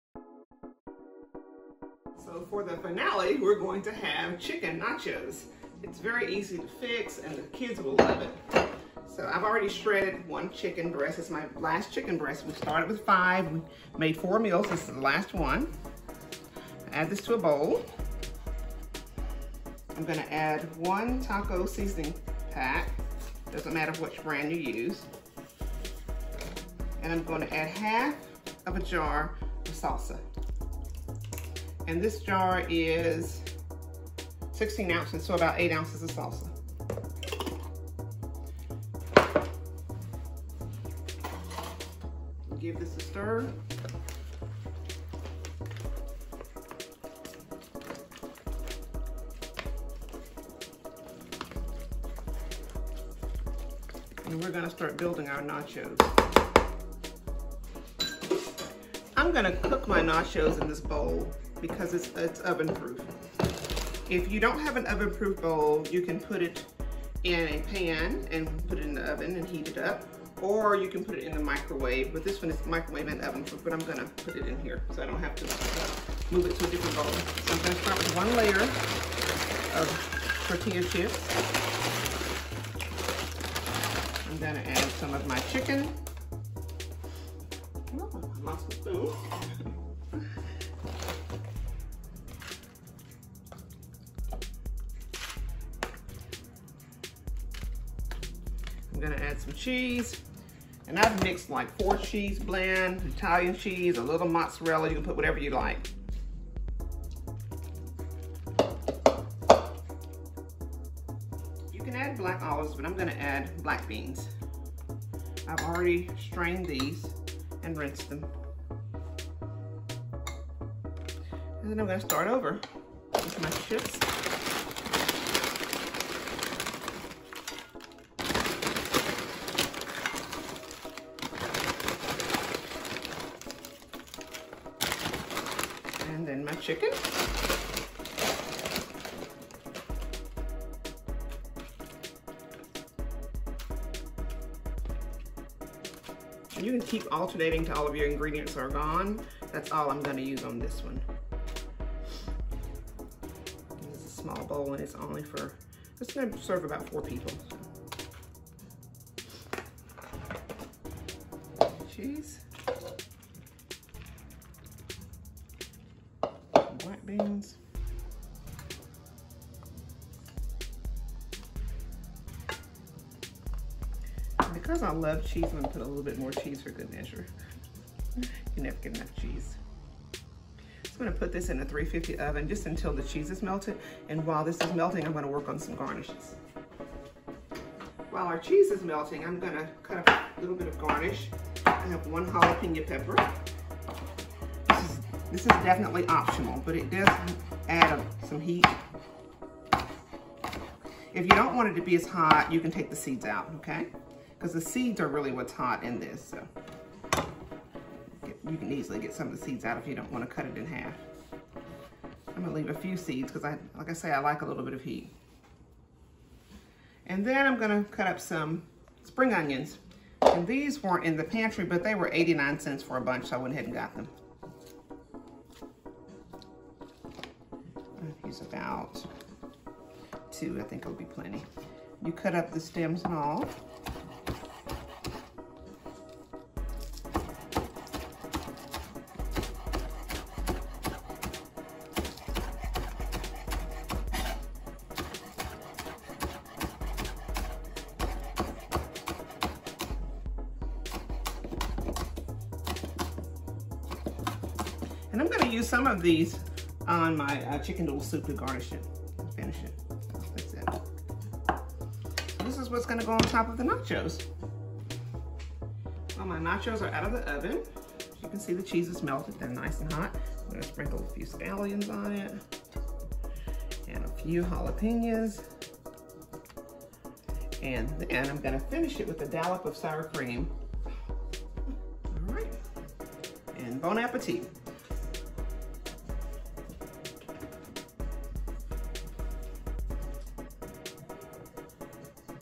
Chicken Nachos Ft. Charlene
Join us as Charlene teaches us how to use the items we have in our Food Pantry to make delicious meals using items from our own PWA Food Pantry!

Recipie:
1 Hot Fresh Shredded Chicken Breast
Taco Seasoning pack
Half Jar of Salza [8oz]
Tortilla Chips